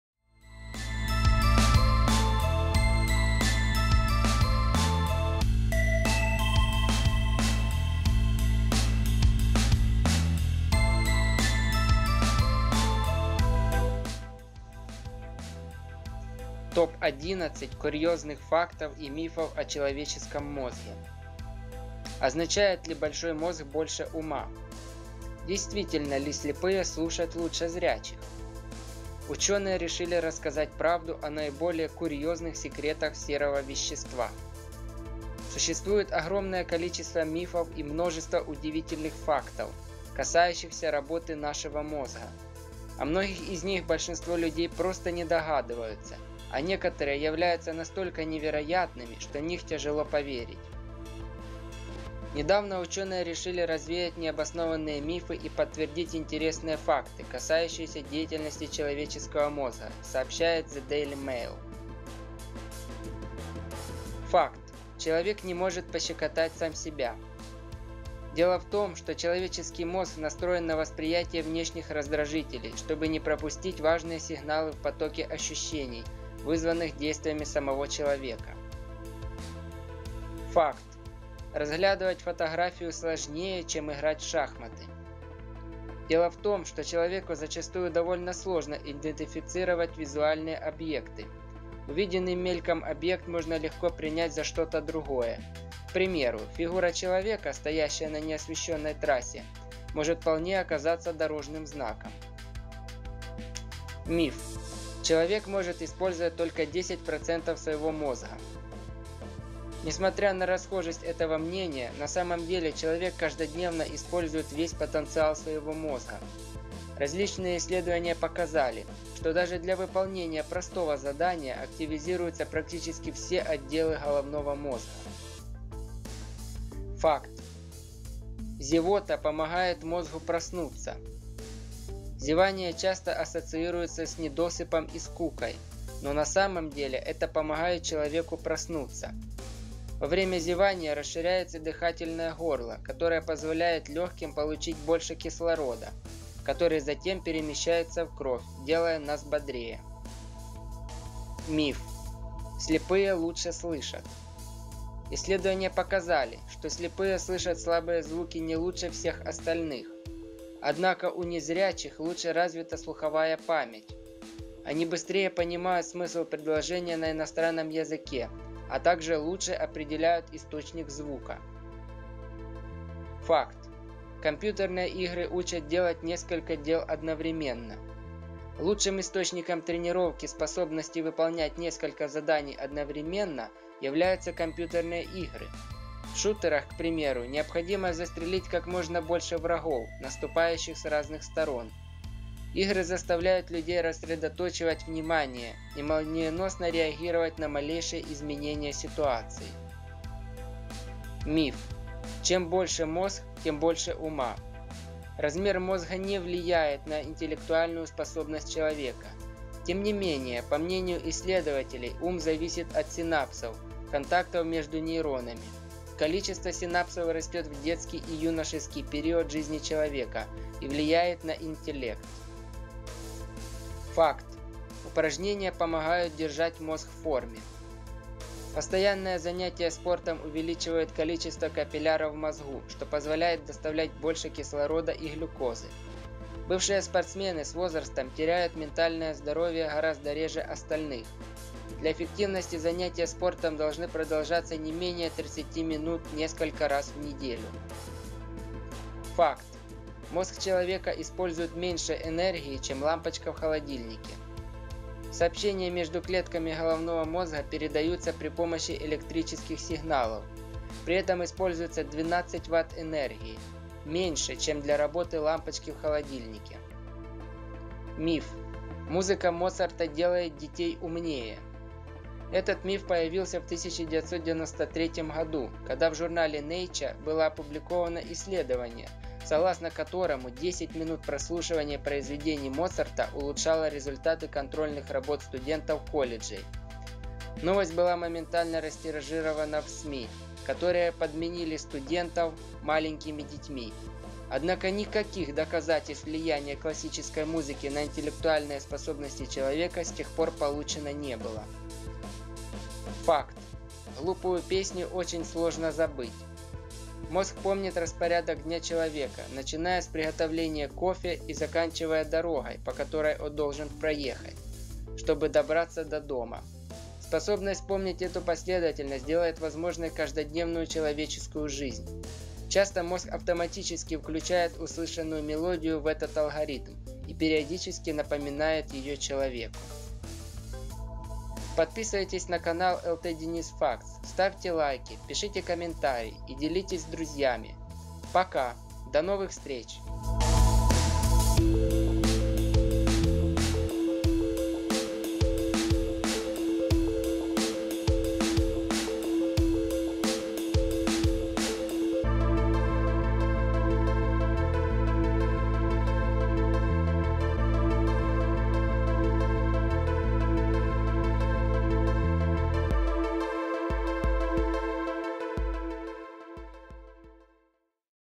Топ-11 курьёзных фактов и мифов о человеческом мозге
Интересные факты. Топ-11 курьёзных фактов и мифов о человеческом мозге

Означает ли большой мозг больше ума? Действительно ли слепые слышат лучше зрячих? Ученые решили рассказать правду о наиболее курьезных секретах серого вещества.
Существует огромное количество мифов и множество удивительных фактов, касающихся работы нашего мозга. О многих из них большинство людей просто не догадываются, а некоторые являются настолько невероятными, что в них тяжело поверить.

Недавно ученые решили развеять необоснованные мифы и подтвердить интересные факты, касающиеся деятельности человеческого мозга.